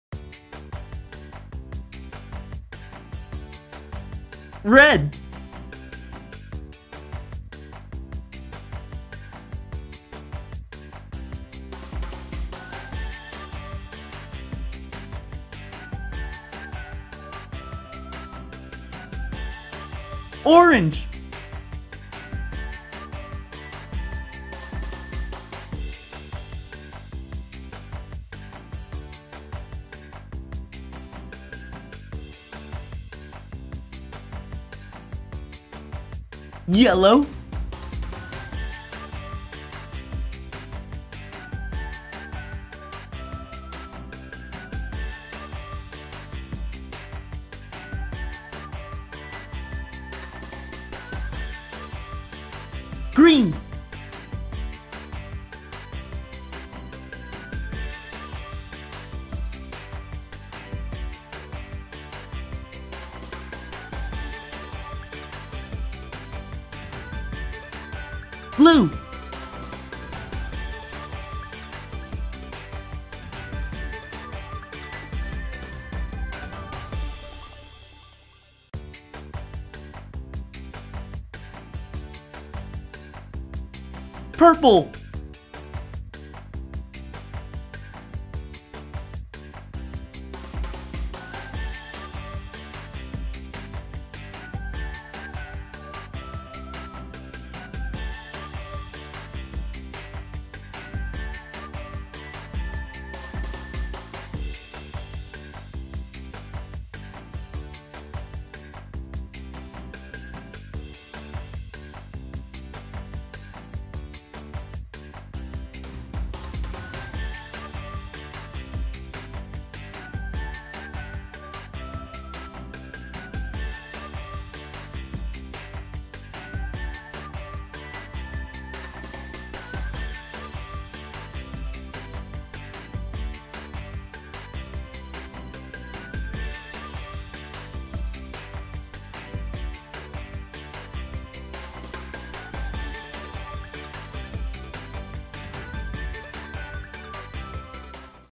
Learn Colors with a Play Doh Rainbow | How To Make Easy!
You'll be able to learn the colors of the rainbow while we show you how to make a Play-Doh Rainbow!